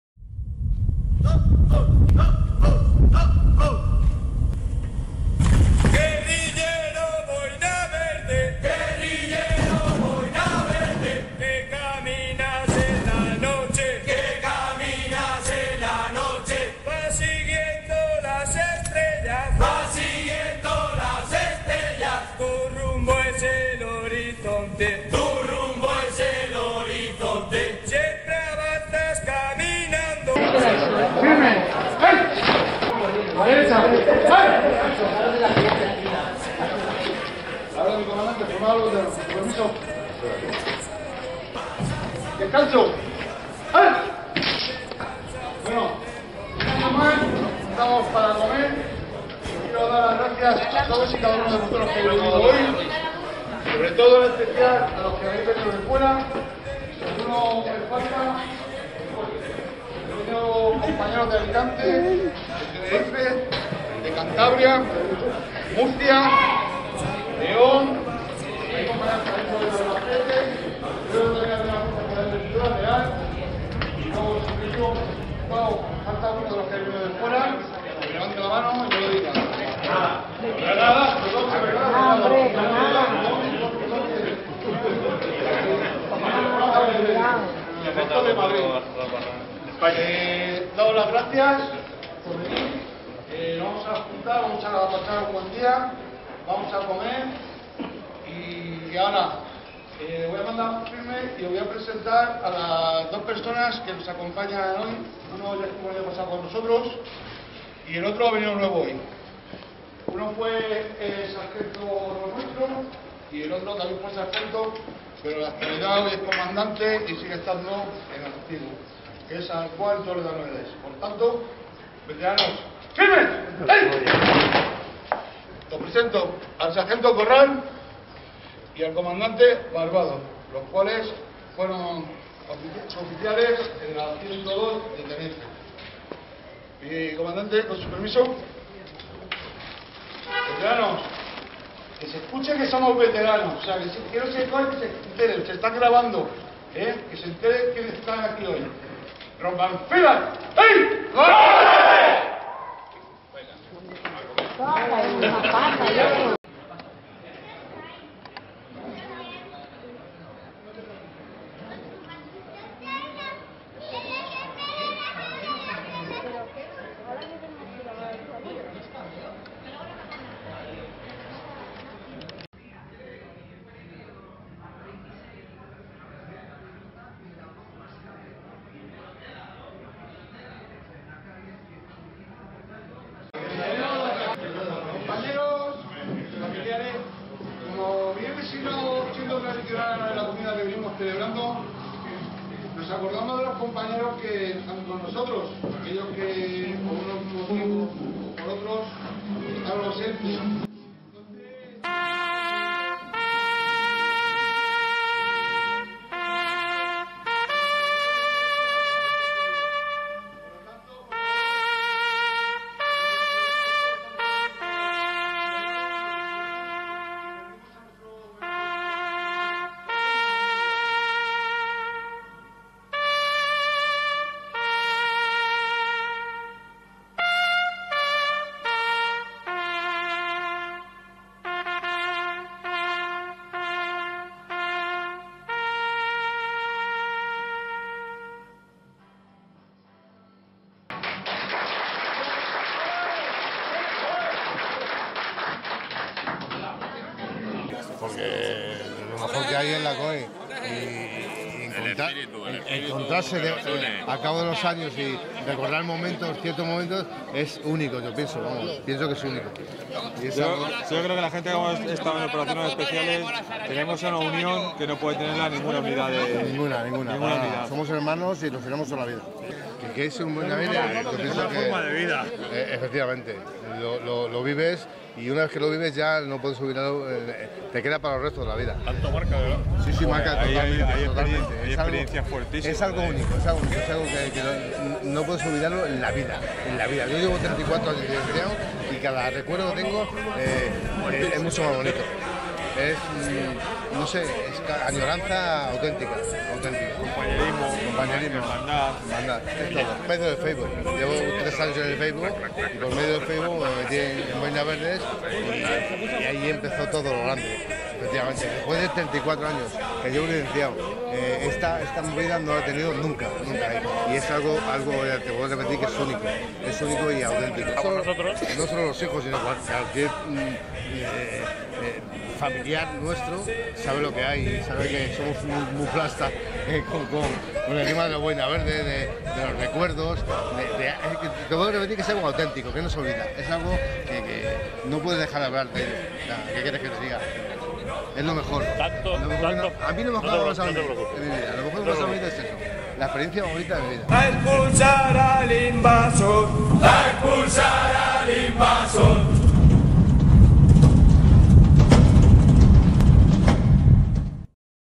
BOINAS VERDES: COMIDA VETERANOS (LEGANÉS)
El sábado 18 de marzo veteranos Boinas Verdes celebraron su tercera comida de reencuentro en Leganés. Llegados de toda España, la cita se ha convertido en un acontecimiento anual al que fue invitado LEGANEWS.